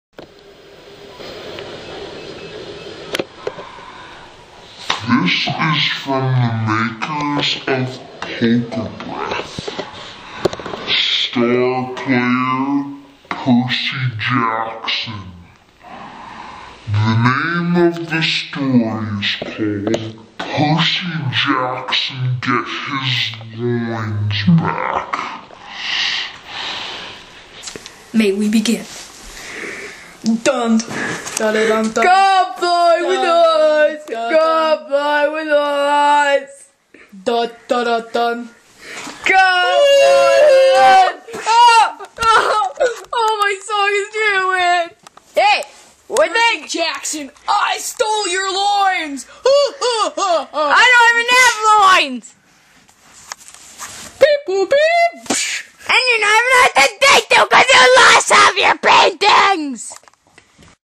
percy jackson gets his loins back! (or does he?)
percy jackson spent over 100 thousand years looking for his loins. but then he forgot about them when he got his pet dragon.